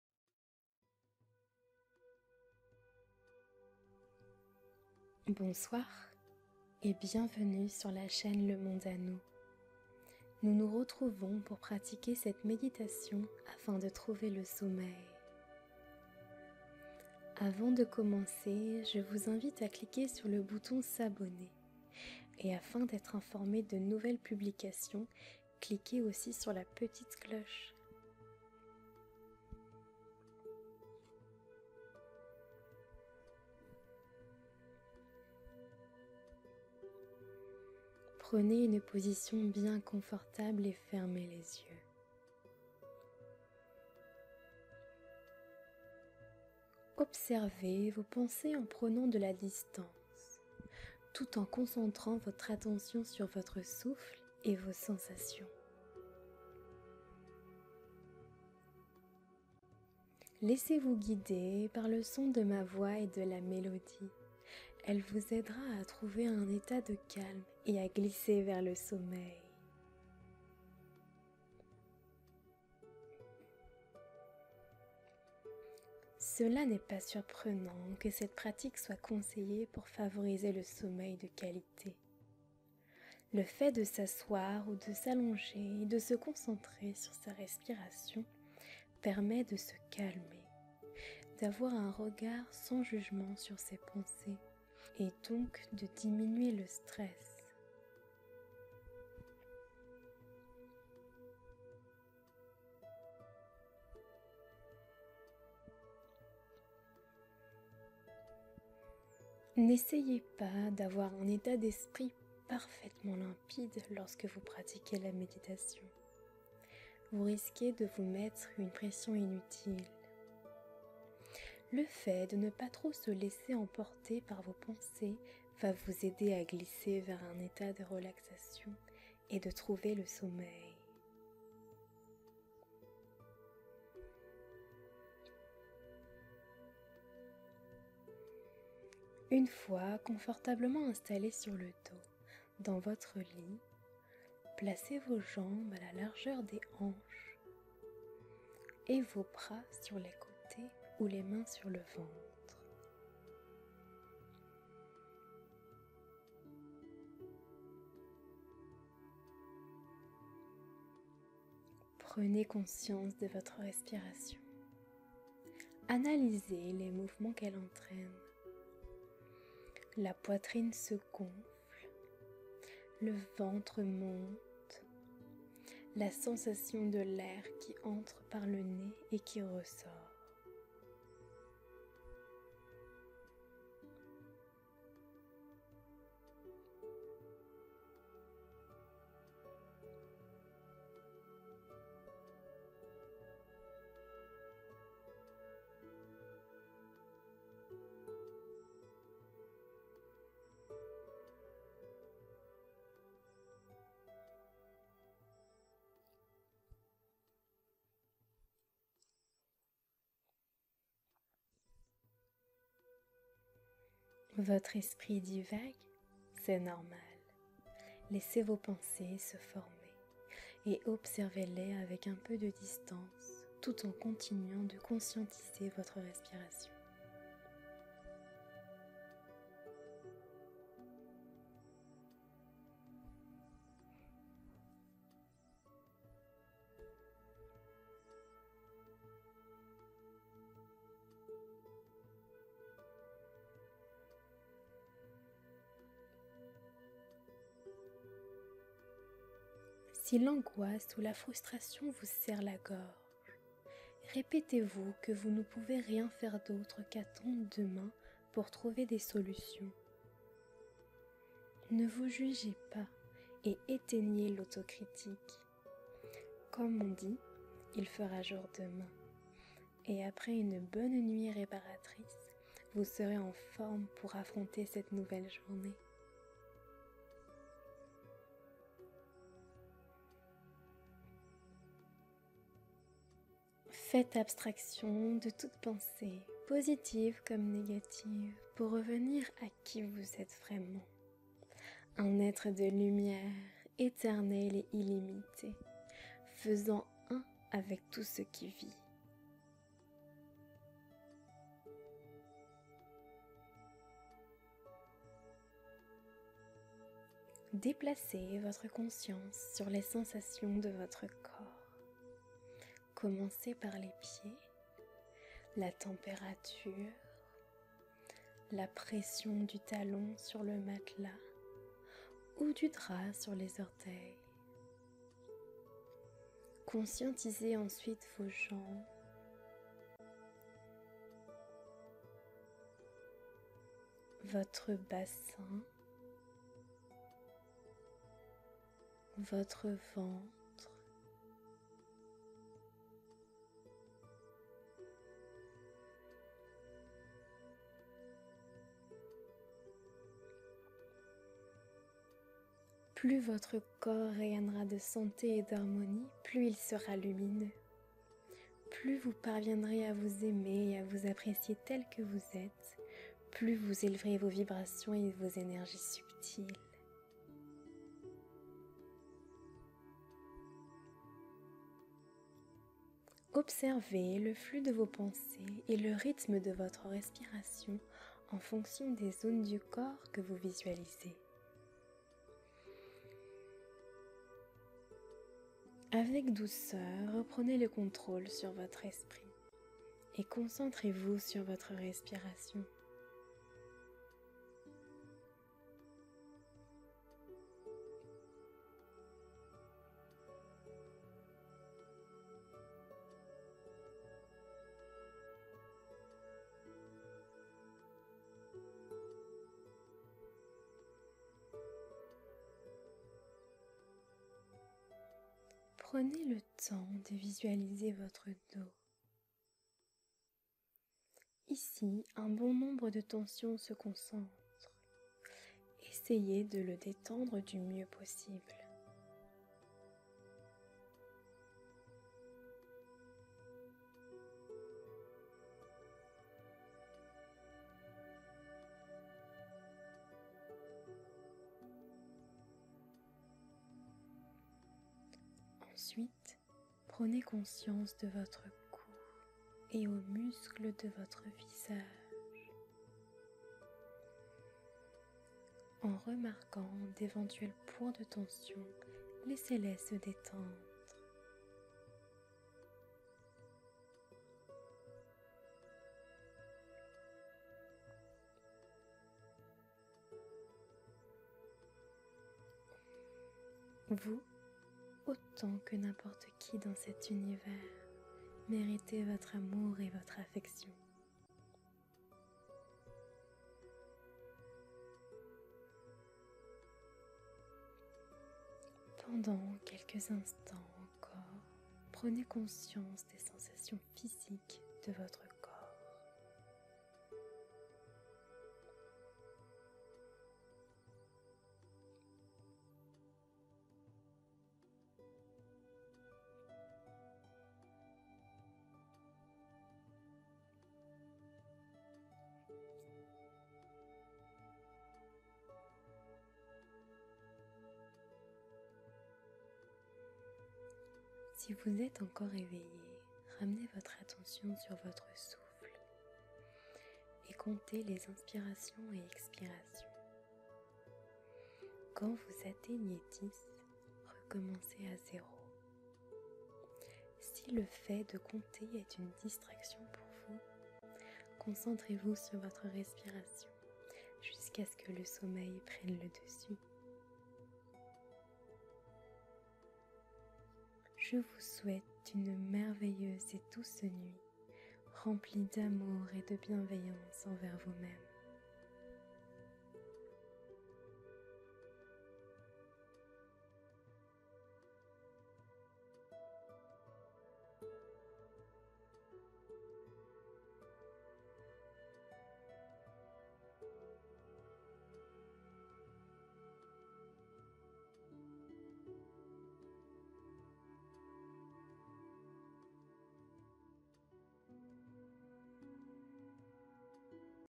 Méditation du Sommeil - Douce Nuit (9 min)
Prenez une position bien confortable et fermez les yeux ..

Faites abstraction de toute pensée, positive comme négative, pour revenir à QUI vous êtes vraiment : un Être de lumière, éternel et illimité, faisant UN avec tout ce qui vit.

RDV TOUS LES JEUDI à 18H 00